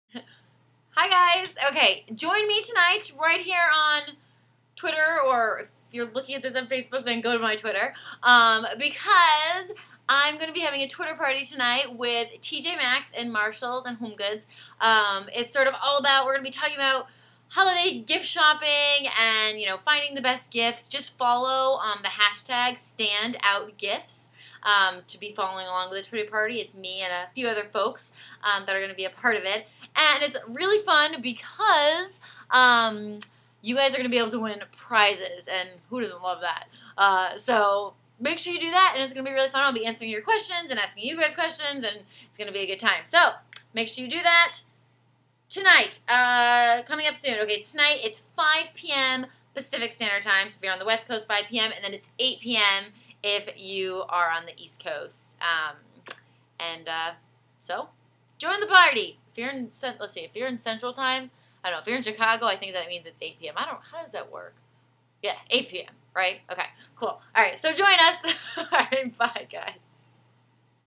Twitter Party TONIGHT!
I am having a Twitter party with TJ Maxx and Marshalls at 5pm PST/8pm EST to talk Holiday gifts! We are giving away great prizes too! So make sure you join the party! Follow standoutgifts to join the party :)